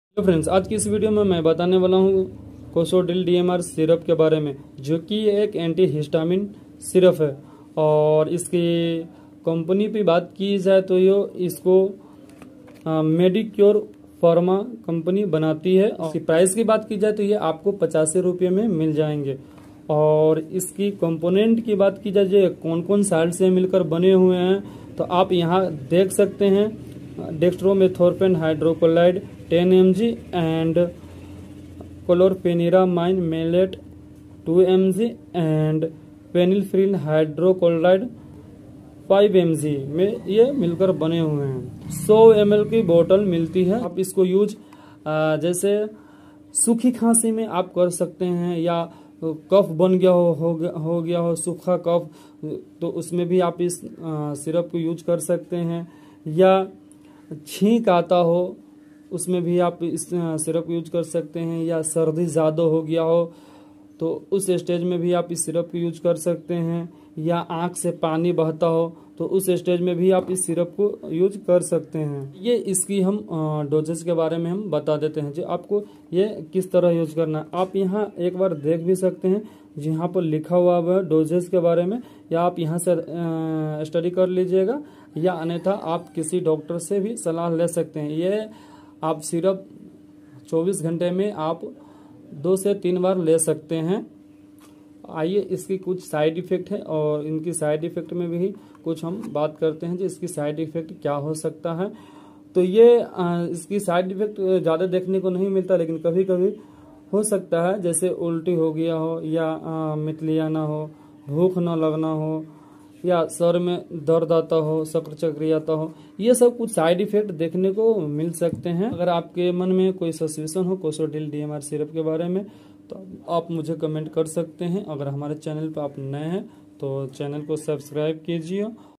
cosodril-DMR syrup use kaise kare cosodril dmr syrup|Dry cough syrup|best cough syrup
Hello Friends
  Aaj ke es video me batane wala hu ki
Cosodril-DMR Syrup bahut achchha cough syrup
jo ki ye dry cough syrup hai sukhi khasi me bahut achchha result hai

Syrup सुखी खांसी, सर्दी, नाक जाम, गला खुजलाना इन सभी बिमारी मे बहुत अच्छा काम करता है
cosodril dmr syrup
Glycodin cough syrup
Ascoril dxsyrup
Ascoril ls syrup
Rexcof DX syrup